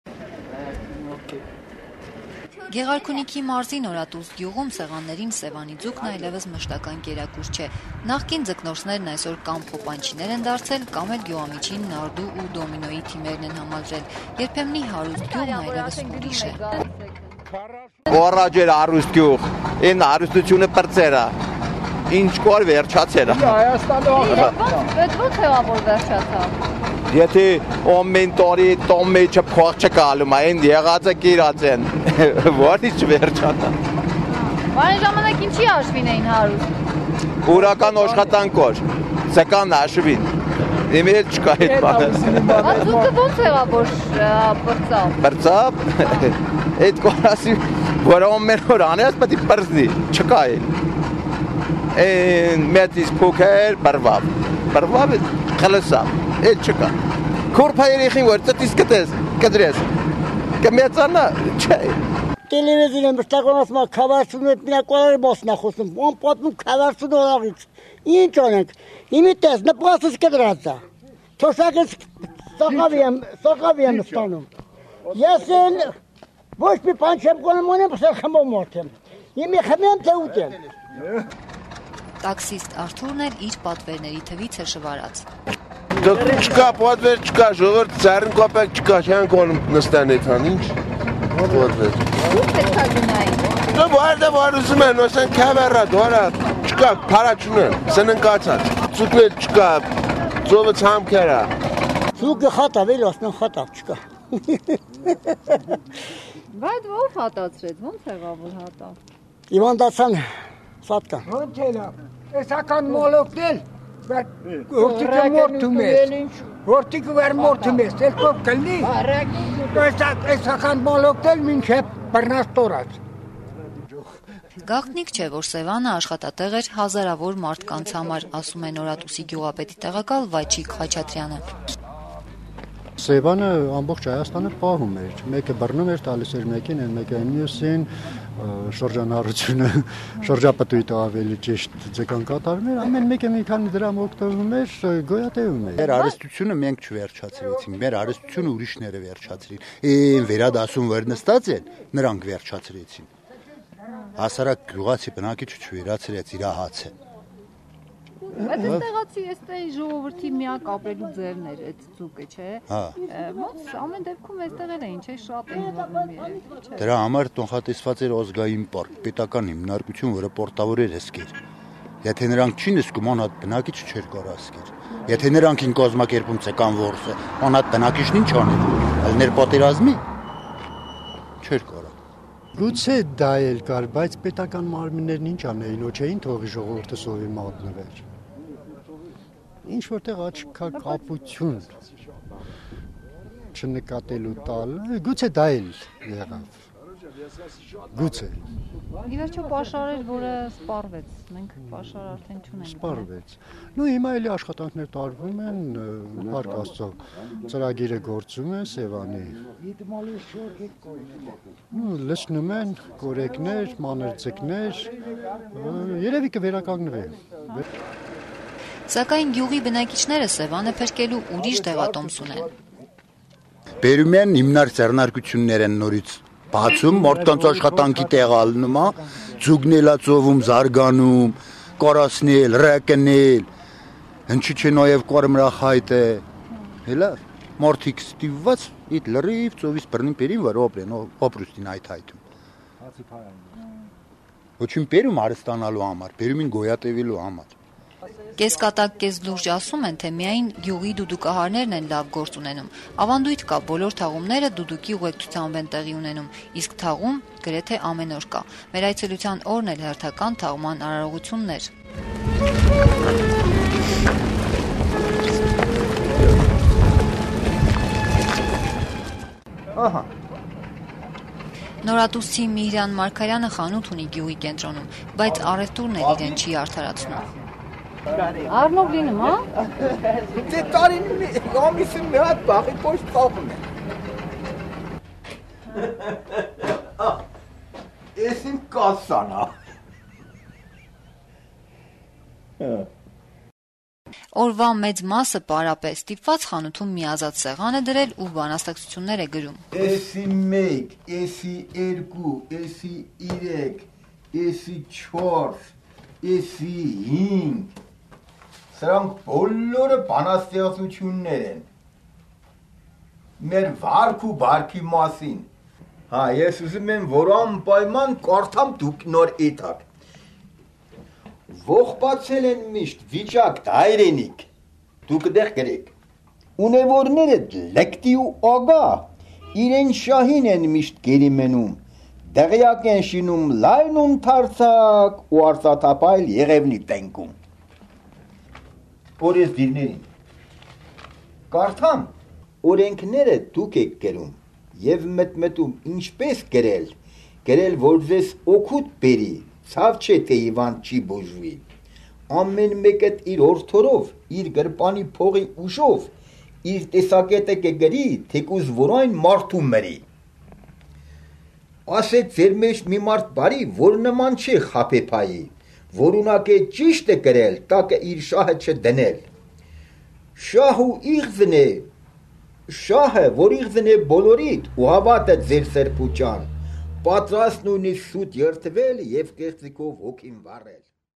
Նախկին ձկնորսներն այլեւս «խոպանչիներ» են
Գեղարքունիքի մարզի Նորատուս գյուղում սեղաններին Սեւանի ձուկն այլեւս մշտական կերակուր չէ: Նախկին ձկնորսներն այսօր կամ խոպանչիներ են դարձել, կամ էլ գյուղամիջին նարդու ու դոմինոյի թիմերն են համալրել: Երբեմնի հարուստ գյուղն այլեւս ուրիշ է: Տեսանյութը` Կարինե Սիմոնյանի: